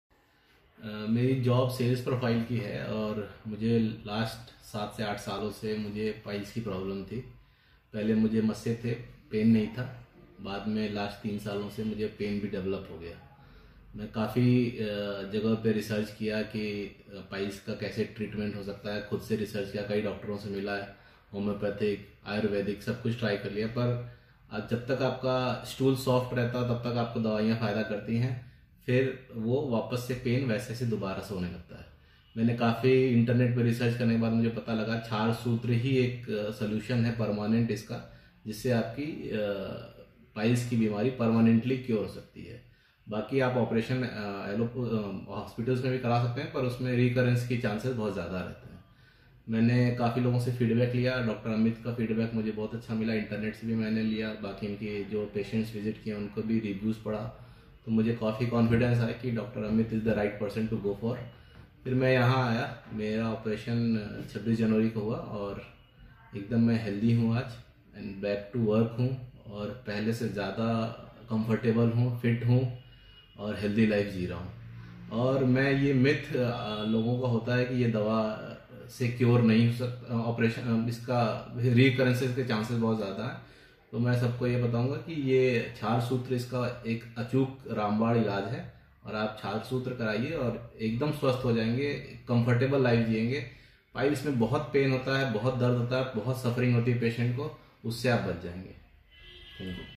India's Best Piles Doctor in Ghaziabad
if you are suffering from piles and you are searching piles doctor in Ghaziabad  then consult in Prakash Medical Centre and get india's best Piles specialist in ghaziabad. If you want to see feedback which given by our patient then just wach this video.